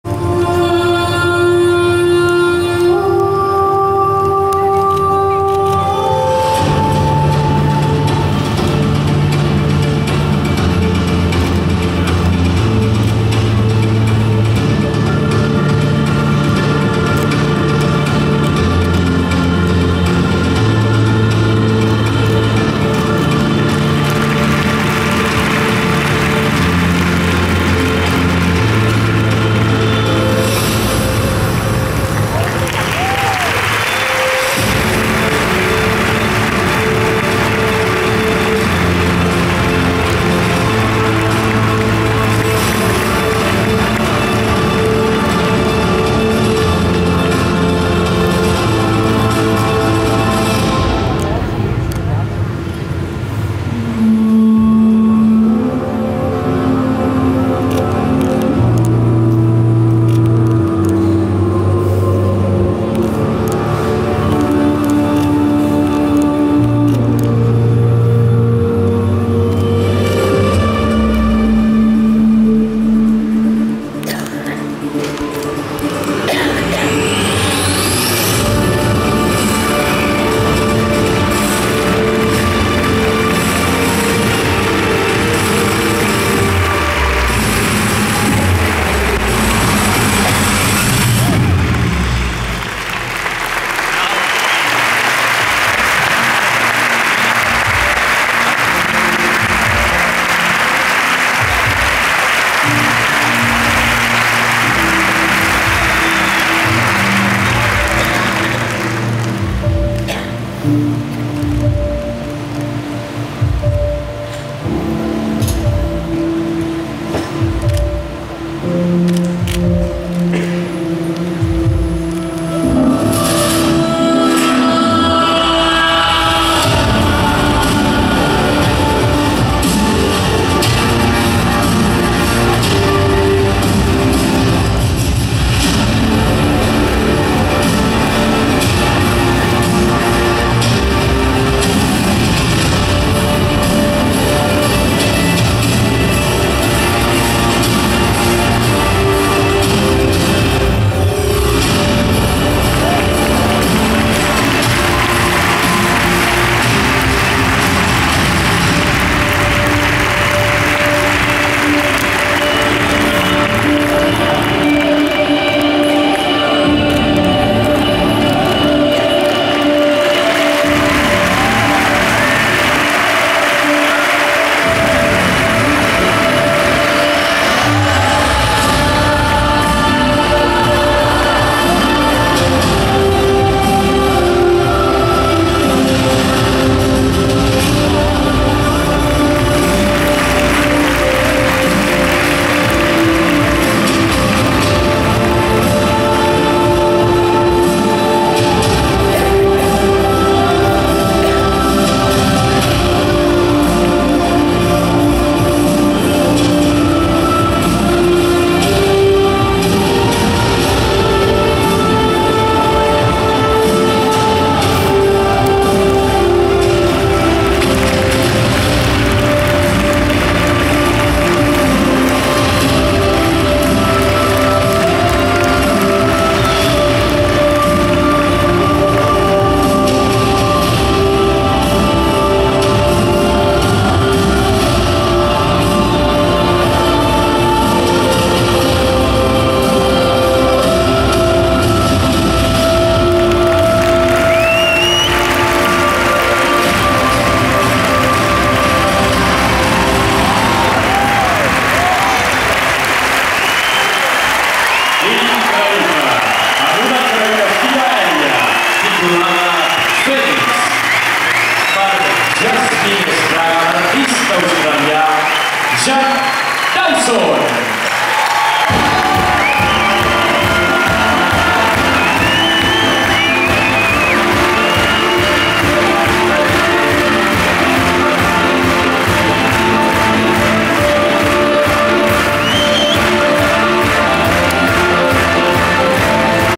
Воздушный гимнаст. Jack Dawson. Teles aèries. Austràlia
7-й Международный цирковой фестиваль в Жироне. Испания